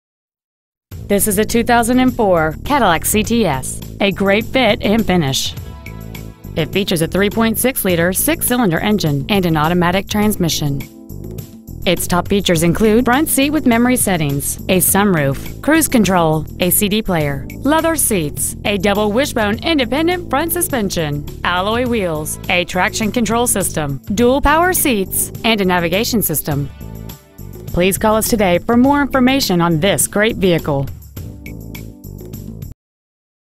2004 Cadillac CTS Franklin TN 37067
Year: 2004
Make: Cadillac
Model: CTS
Engine: 3.6L V6
Trans.: AUTO
Exterior: Black
Miles: 88,955